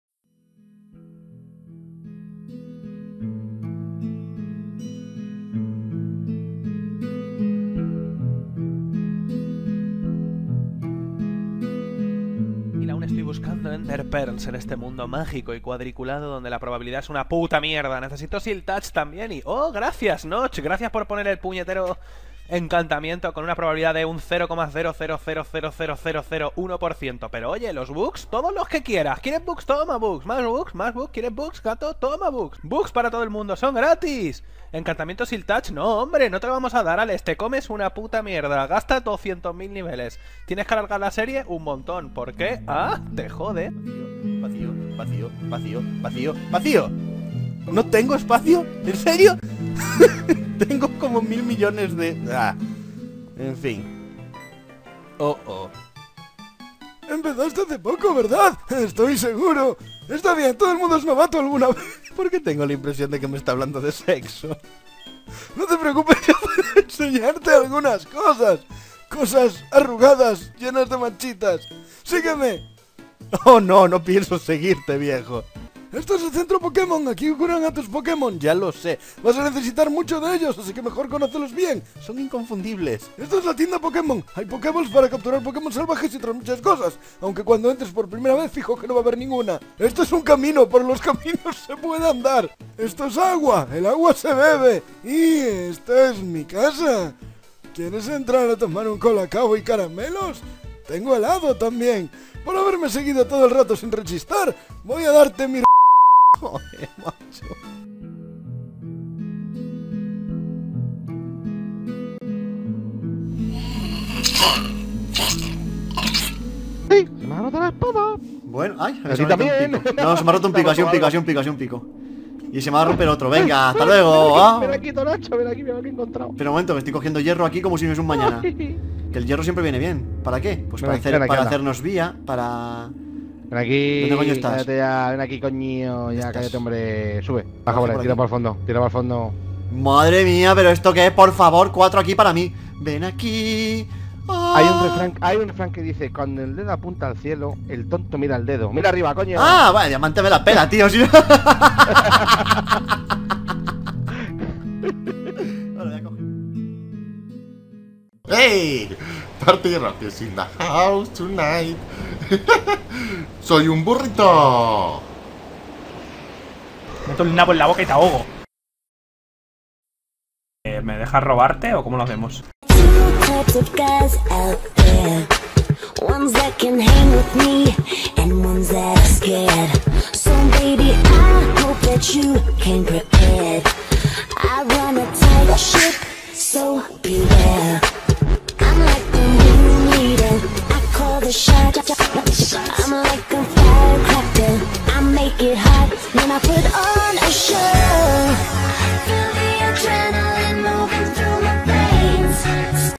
AS 01 TRAILER Alexelcapo, Rangu, Chincheto, Tonacho, helldogmadness
Me gustaría hacer una serie de vídeos de animación con youtubers de todo tipo. Este vídeo lo uso como maqueta y trailer para que sepais como se verá. Si querios otro youtuber, escribidme por comentarios. Si además me mandais momentos graciosos, mejor que mejos.
ciao ;)